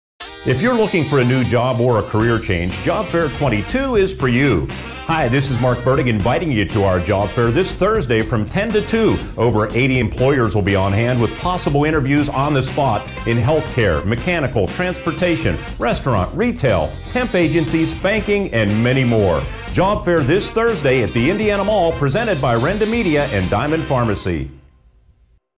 Job Fair 2022
More than 80 employers will be on hand to meet you! Renda Media is excited to offer you the opportunity of a lifetime! Your new career awaits you at Job Fair 2022, presented by Diamond Pharmacy. Save the date for Thursday, March 31 from 10-2 at the Indiana Mall. Bring your resume, come dressed for success, and meet employers wanting to meet you! Possibly interviews on the spot with employers in healthcare, mechanical, transportation, restaurant, retail, temp agencies, banking and many more!